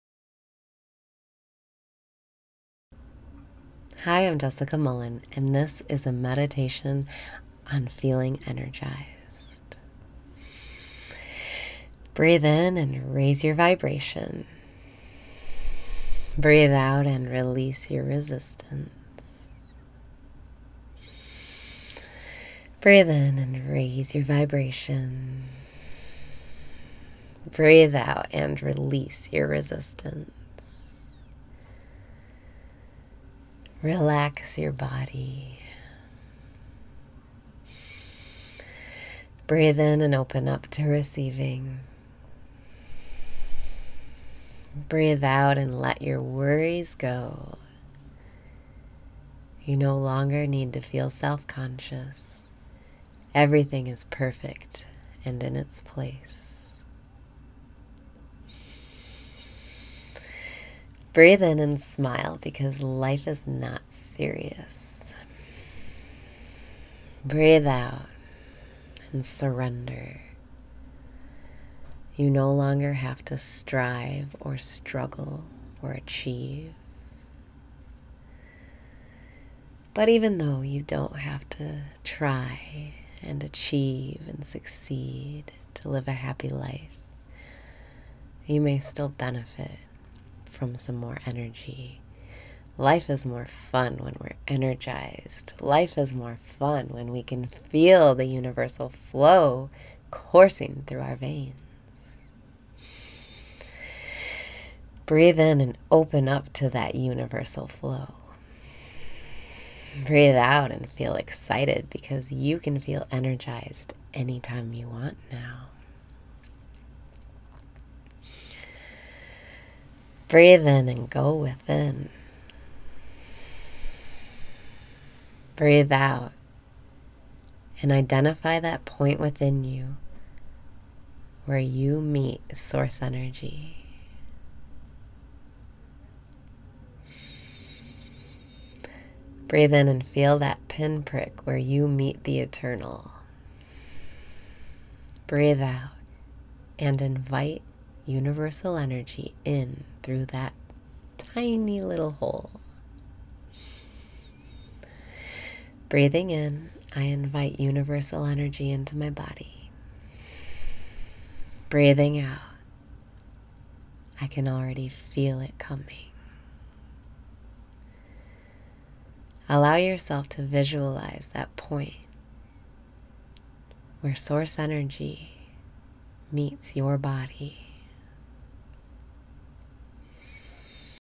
Meditation on Feeling Energized [PREVIEW]
This is a preview of a 15 minute meditation video available for sale or subscriber download at

You no longer need to feel self-conscious. Everything is perfect and in its place.

Breathe in and smile because life is not serious!

You no longer have to strive or struggle or achieve. But even though you don’t have to try and achieve and succeed to live a happy life, you may still benefit from some more energy. Life is more fun when we’re energized. Life is more fun when we can feel the universal flow coursing through our veins.

Breathe in and open up to that universal flow.
Breathe out and feel excited because you can feel energized anytime you want now.

Breathe in and go within.
Breathe out and identify that point within you where you meet source energy.

Breathe in and feel that pinprick where you meet the eternal.
Breathe out and invite universal energy in through that tiny little hole.

Breathing in I invite universal energy into my body.
Breathing out I can already feel it coming.

Allow yourself to visualize that point, where source energy meets your body...